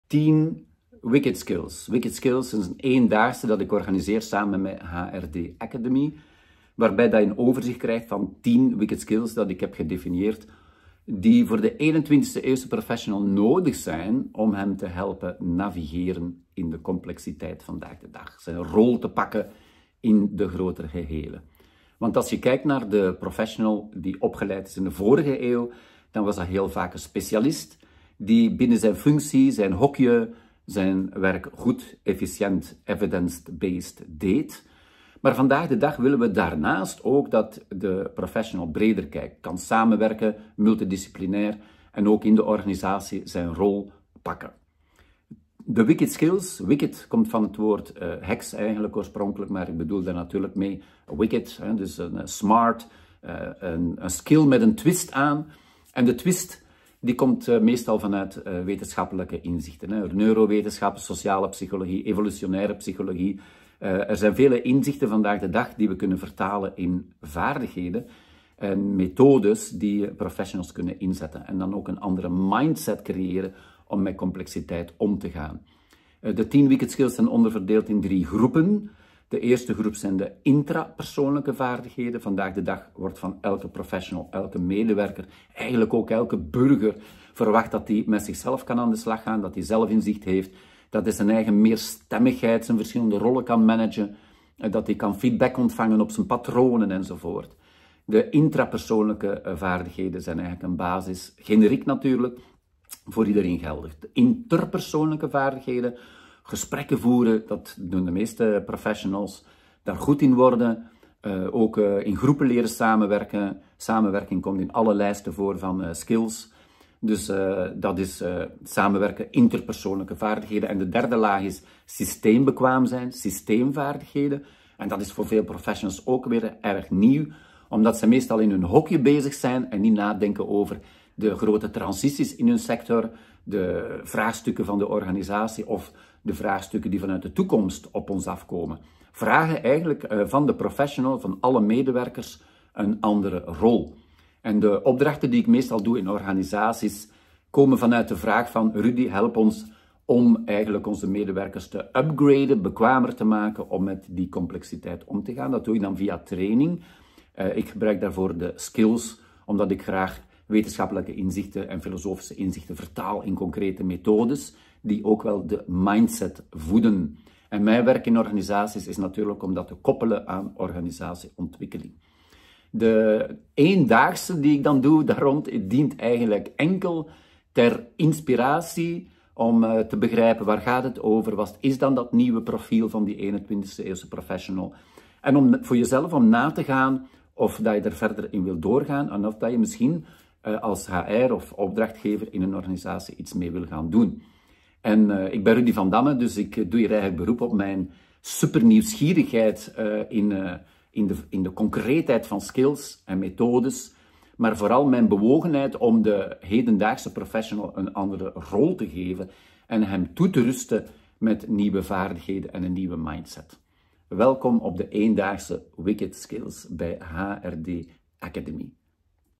Vandamme. De ééndaagse Wicked Skills
Wicked Skills helpen je als professionals om beter te worden je weg te vinden in een complexe wereld. In deze korte video geef ik uitleg over de 3 niveaus in de Wicked Skills en wat je mag verwachten op de ééndaagse, georganiseerd door @HRD_Academy